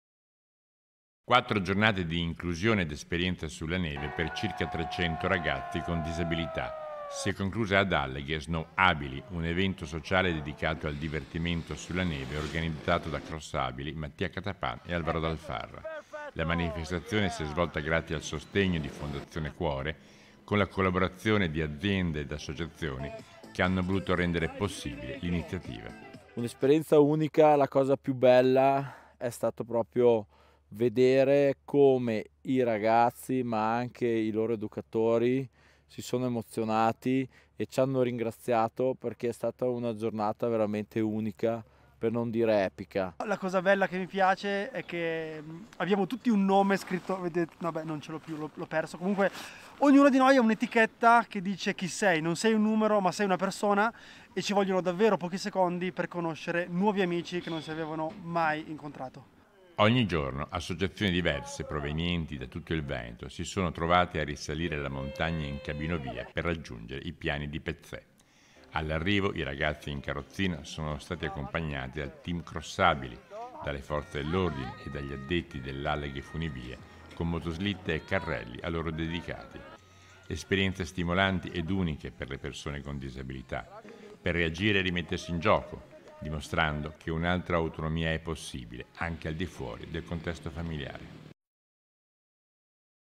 QUATTRO GIORNATE SULLA NEVE ALL'INSEGNA DELL'INCLUSIONE | 18/02/2023
A3 NEWS Treviso 18/02/2023 - ALLEGHE - Motoslitte, snow-bike, balli, musica, pranzo in baita e un giro in cabinovia, queste le attività svoltesi all'interno di "SNOW-abili", un evento sociale dedicato all'inclusione e al divertimento sulla neve.Vediamo di cosa si tratta.  || Quattro giornate di inclusione ed esperienze sulla neve per circa 300 ragazzi con disabilità. Si è conclusa ad Alleghe "SNOW-abili", un evento sociale dedicato al divertimento sulla neve organizzato da CROSSabili, Mattia Cattapan e Alvaro Dal Farra.La manifestazione si è svolta grazie al sostegno di Fondazione Cuore con la collaborazione di aziende e associazioni che hanno voluto rendere possibile l'iniziativa.Ogni giorno associazioni diverse provenienti da tutto il Veneto si sono trovate a risalire la montagna in cabinovia per raggiungere i Piani di Pezzè. All’arrivo i ragazzi in carrozzina sono stati accompagnati dal team CROSSabili, dalle Forze dell'Ordine e dagli addetti dell'Alleghe Funivie con motoslitte e carrelli a loro dedicati. Esperienze stimolanti ed uniche per le persone con disabilità: per reagire e rimettersi in gioco, dimostrando che un'altra autonomia è possibile anche al di fuori del contesto familiare. - Intervistati ALVARO DAL FARRA (CROSS-ABILI) (Servizio di Fabio Fioravanzi) - Segui Antenna Tre anche sul digitale terrestre! Visita il sito